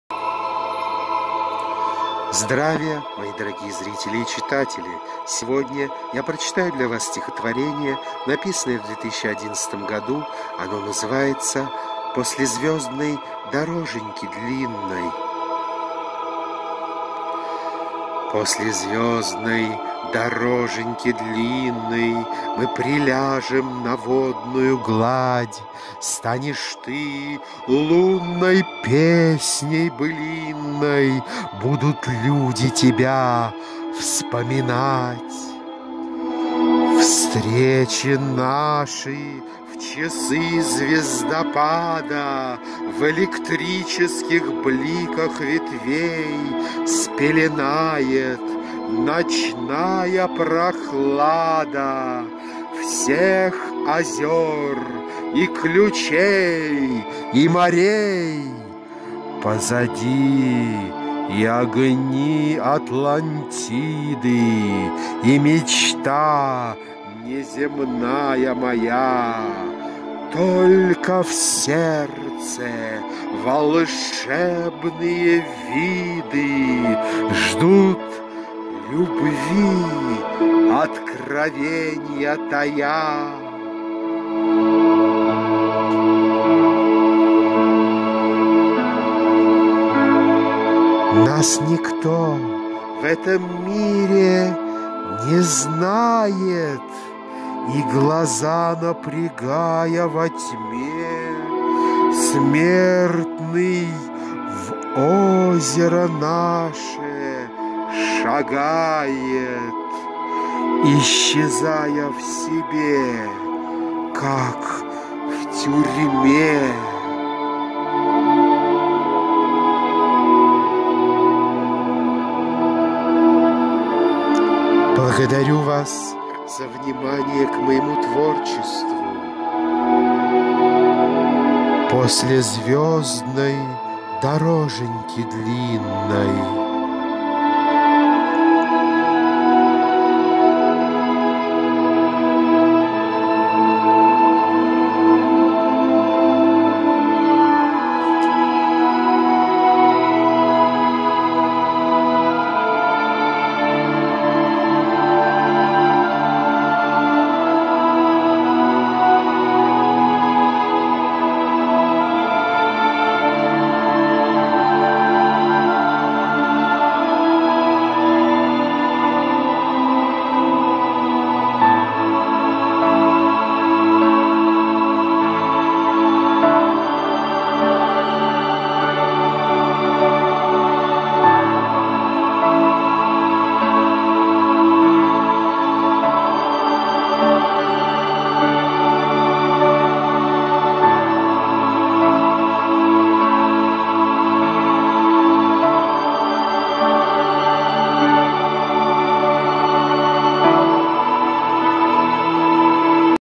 "После звёздной дороженьки длинной..." - стихи о любви и вечности в авторском исполнении
После звёздной дороженьки длинной...

Мы приляжем на водную гладь,
Станешь ты лунной песней былинной,
Будут люди тебя вспоминать...

Встречи наши в часы звездопада,
В электрических бликах ветвей
Спеленает ночная прохлада
Всех озёр и ключей, и морей...

Позади и огни Атлантиды,
И мечта неземная моя,
Только в сердце волшебные виды
Ждут любви, откровенья тая...

Нас никто в этом мире не знает,
И, глаза напрягая во тьме,
Смертный в озеро наше шагает,
Исчезая в себе, как в тюрьме!...

                                               19.03.11